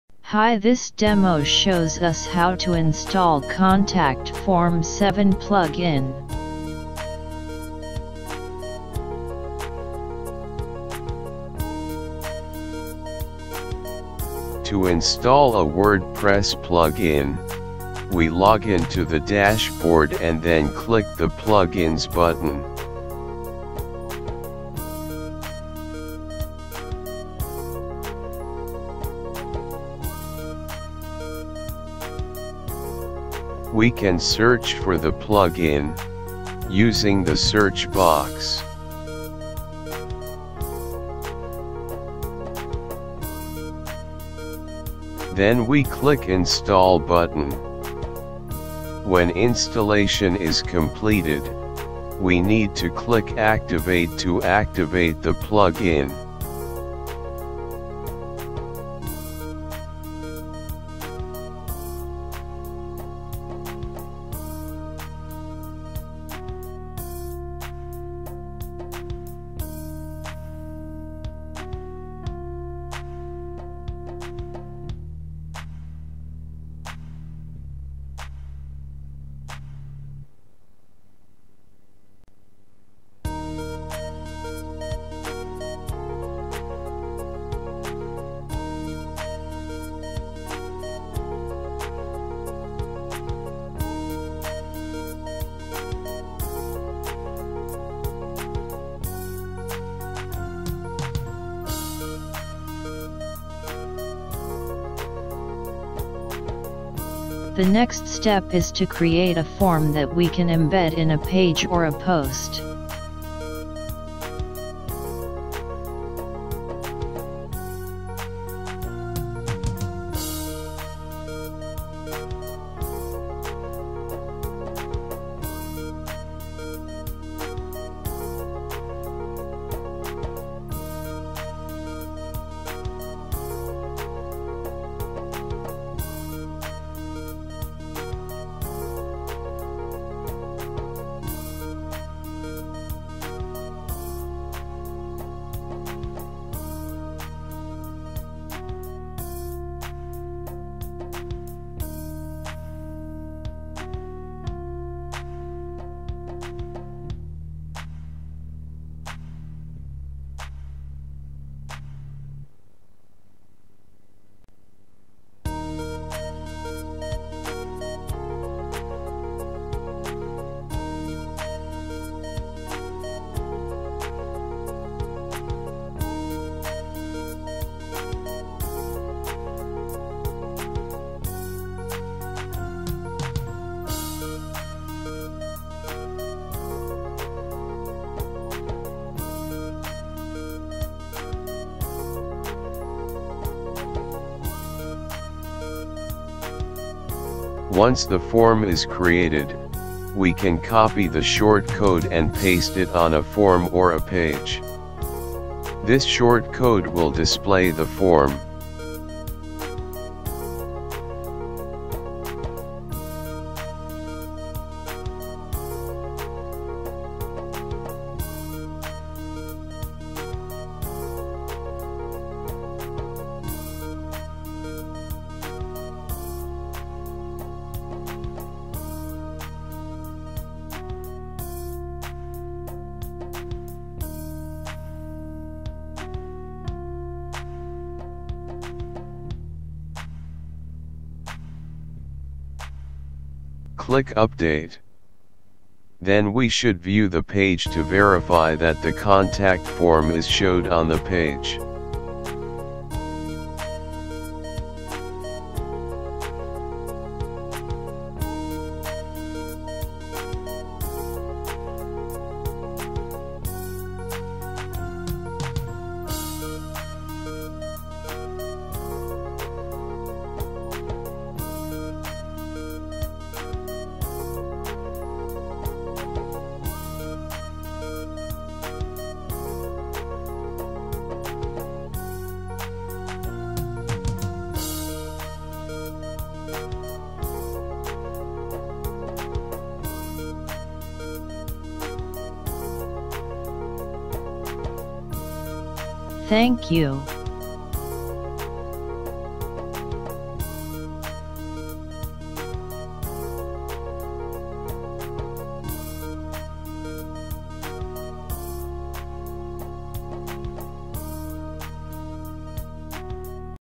Word demo install contact form 7 plugin
This demo shows how to install Contact Form 7 Plugin, a WordPress demo. Then shows how to create a quick basic contact form and use this form in a page. This demo is for WordPress beginner